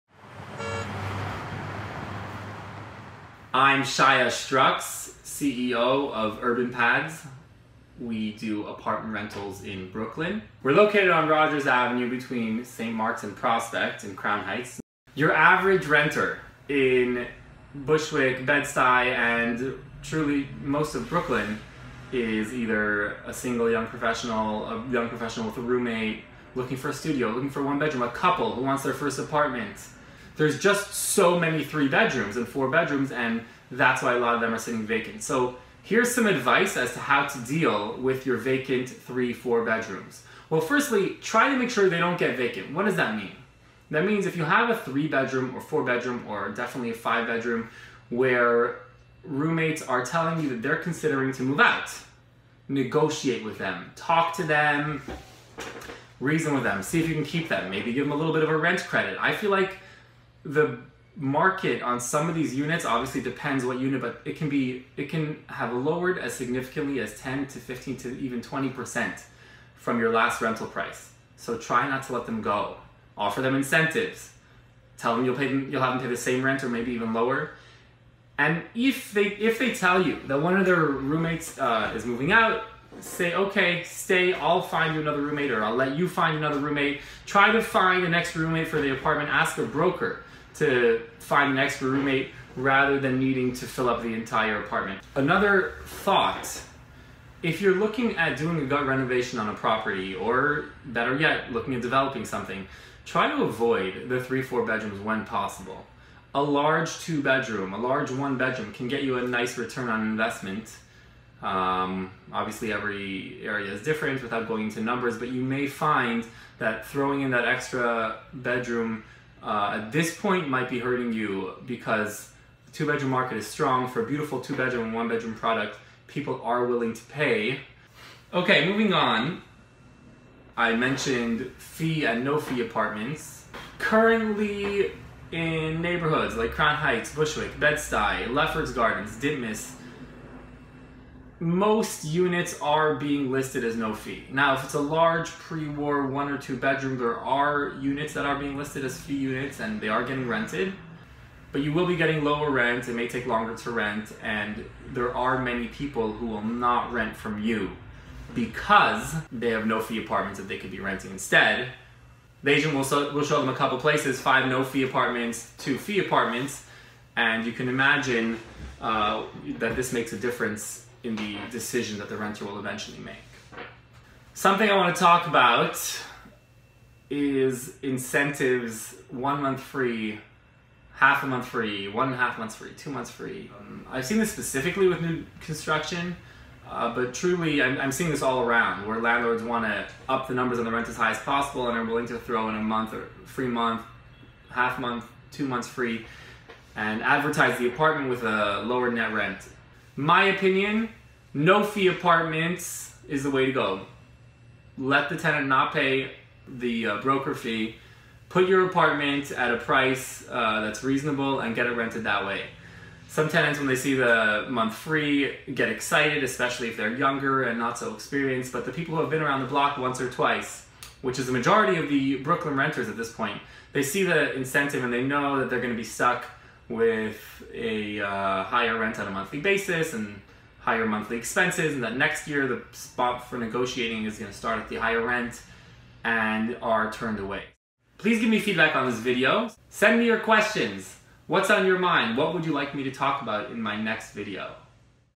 Get Your Apartments Rented! Part 1 HQ
Tips for Brooklyn landlords.  Shaya Schtroks CEO of Urban Pads.